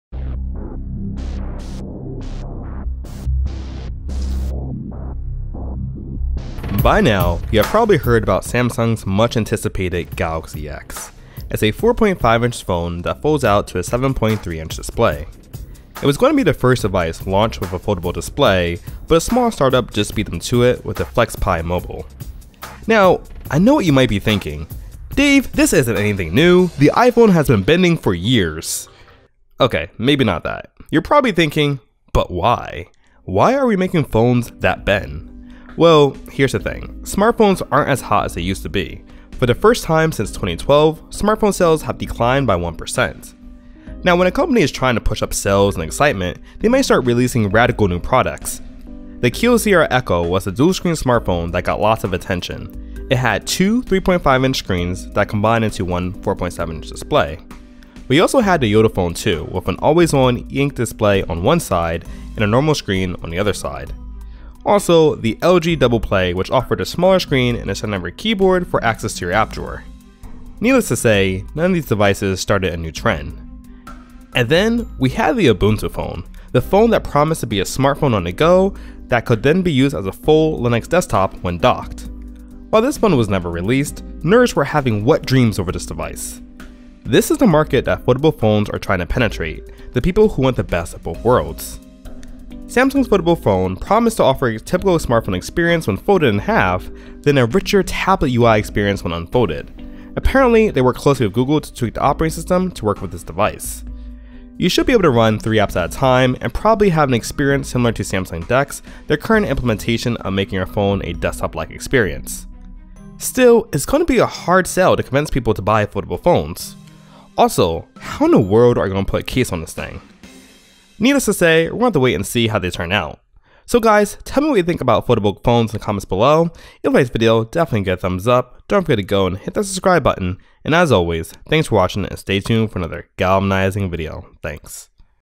This is the Foldable Smartphone
Want to close your phone angrily like how you did back with flip phones? Now you can! Just be a little more careful with it. Samsung and many others are working on Foldable smartphones. The Galaxy X is Samsung's much anticipated foldable phone. So how did we get here?